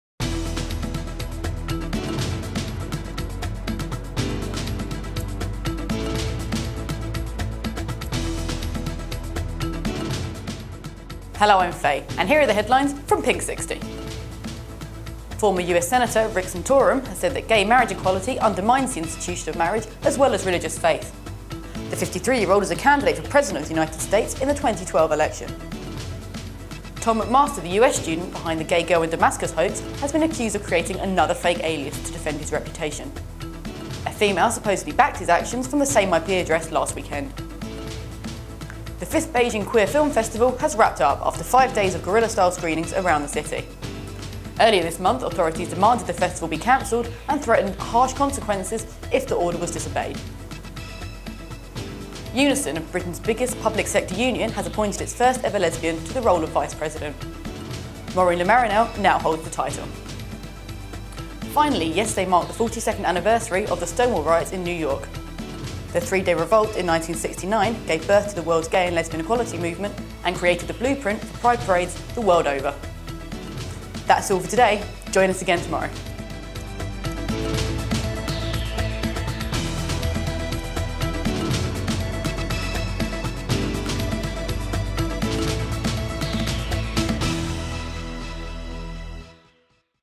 Pinksixty WEDNESDAY 29 JUNE 2011
Former U.S. senator Rick Santorum has said that gay marriage equality undermines the institution of marriage as well as religious faith. The 53 year-old is a candidate for President of the United States in the 2012 election.

Tom MacMaster, the US student behind the Gay Girl in Damascus hoax, has been accused of creating another fake alias to defend his reputation. An alleged female supported MacMaster's actions from the same IP address last weekend.

The fifth Beijing Queer Film Festival has wrapped up after five days of guerrilla-style screenings around the city. Earlier this month, authorities demanded the festival be cancelled and threatened "harsh consequences" if the order was disobeyed.

UNISON -- Britain's biggest public sector trade union -- has appointed its first-ever lesbian to the role of Vice President. Maureen Le Marinel, branch secretary for UNISON's Lancashire Police branch, now holds the title.

Finally, yesterday marked the 42nd anniversary of the Stonewall riots in New York City. The three-day revolt in 1969 gave birth of the world's gay and lesbian equality movement and created the blueprint for Pride parades the world over.